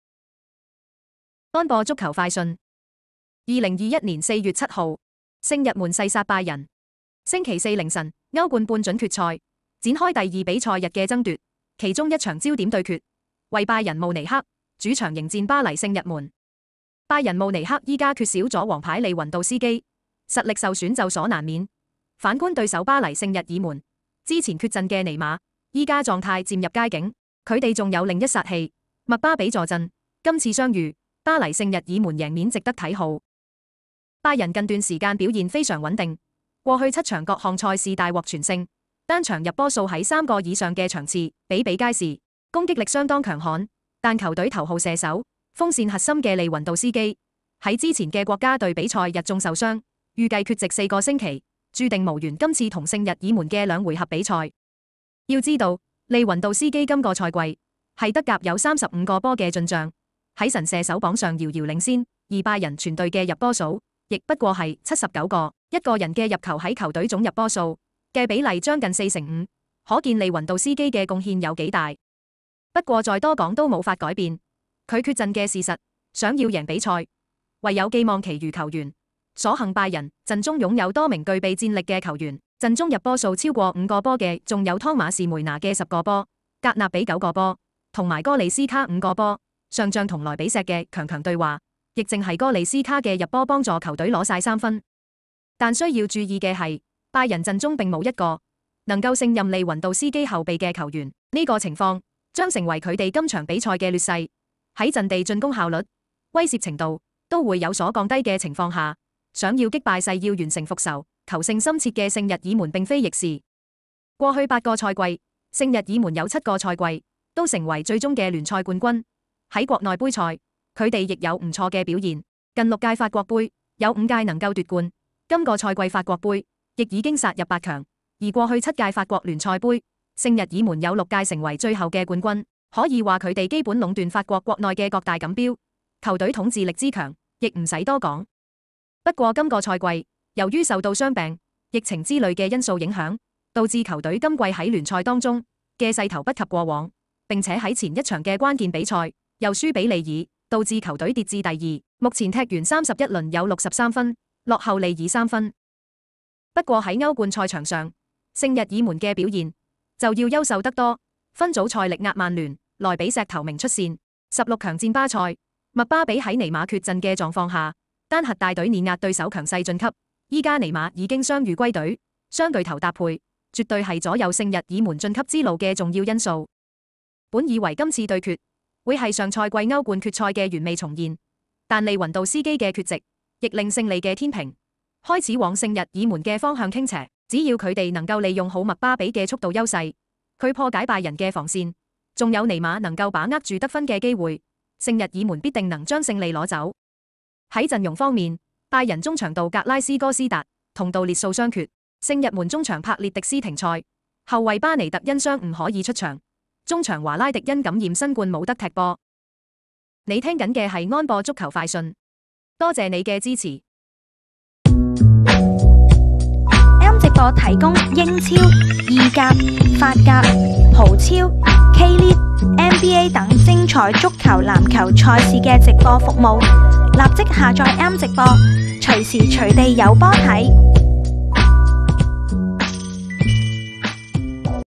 【歐冠盃-賽前新聞】2021-04-08 拜仁慕尼黑 VS 巴黎聖日門 | 聖日門誓殺拜仁
週四凌晨，歐冠 半準決賽展開第二比賽日的爭奪，其中一場焦點對決，為#拜仁慕尼黑 主場迎戰#巴黎聖日門 。拜仁慕尼黑如今缺少了王牌利雲度斯基，實力受損在所難免，反觀對手巴黎聖日門，此前缺陣的尼馬如今狀態漸入佳境，且他們還有另一殺器麥巴比坐鎮，此番相遇，巴黎聖日門贏面值得秤先。

拜仁近段時間表現非常穩定，過去7場各項賽事大獲全勝，單場進球數在3球以上的場次比比皆是，攻擊力相當強悍，但球隊頭號射手、鋒線核心的利雲度斯基在此前的國家隊比賽日中受傷，預計缺席四週，註定無緣本輪與聖日門的兩回合比賽。要知道，利雲度斯基本賽季在德甲有35球進賬，在神射手榜上遙遙領先，而拜仁全隊的入球數也「不過是」79球，一個人的入球在球隊總入球數的佔比將近四成五，可見利雲度斯基的貢獻有多麼大。不過說再多也沒辦法改變此子缺陣的事實，想要贏下比賽，唯有寄望其餘球員，所幸拜仁陣中擁有多名具備戰力的球員，陣中入球數超過5球的還有湯馬士梅拿(10球)、格納比(9球)以及哥利斯卡(5球)，上仗與RB萊比錫的強強對話，亦正是哥利斯卡的入球幫助球隊全取3分。但需要注意的是，拜仁陣中並沒有一名能夠勝任利雲度斯基後備的球員，這將成為他們本場比賽的劣勢，在陣地進攻效率、威懾程度都會有所降低的情況下，想要擊敗誓要完成復仇、求勝心切的聖日門並非易事。

過去8個賽季，聖日門有7個賽季都成為最終的聯賽冠軍，國內盃賽他們也有不俗的表現，近6屆法國盃有5屆能夠奪冠，本賽季法國盃也已殺入八強，而過去7屆法國聯賽盃，聖日門有6屆成為最後的冠軍，可以說他們基本壟斷法國國內的各大錦標，球隊統治力之強也無需多言。不過本賽季，由於受到傷病、疫情之類的因素影響，導致球隊今季在聯賽當中的勢頭不及過往，並且在前一場的關鍵比賽又敗給利爾，導致球隊跌至第二，目前31輪戰罷積63分，落後利爾3分。不過在歐冠賽場上，聖日門的表現則要優秀不少，分組賽力壓曼聯、RB萊比錫頭名出線；十六強戰巴塞，麥巴比在尼馬缺陣的狀況下單核帶隊碾壓對手強勢晉級，如今尼馬已經傷癒歸隊，雙巨頭搭配絕對是左右聖日門晉級之路的重要因素。

本以為今次對決會是上賽季歐冠決賽的原味重現，但利雲度斯基的缺席也讓勝利的天秤開始往聖日門的方向傾斜，只要他們能夠利用好麥巴比的速度優勢去破解拜仁的防線，並且尼馬能夠把握住得分機會，聖日門必定能將勝利帶走。

陣容方面，拜仁中場杜格拉斯哥斯達和杜列素傷缺，而前鋒格納比感染新冠無法出戰。聖日門中場柏列迪斯停賽，後衛巴尼特和前鋒伊卡迪等人因傷缺席，中場華拉迪和費奧倫斯因感染新冠缺陣。